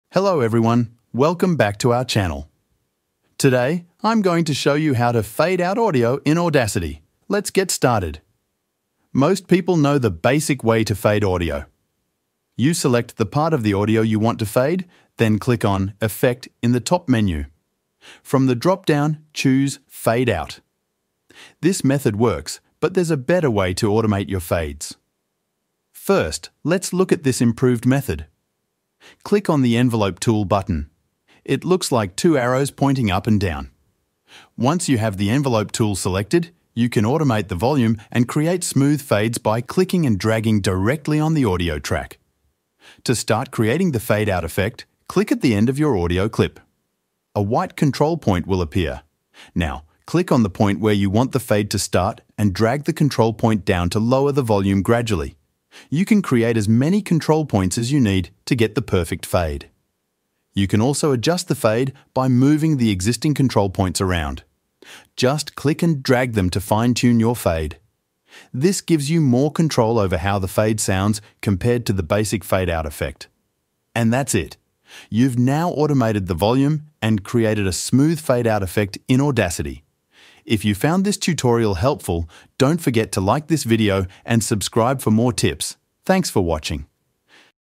How to Fade Out in Audacity
In this quick tutorial, I show you how to fade audio in Audacity.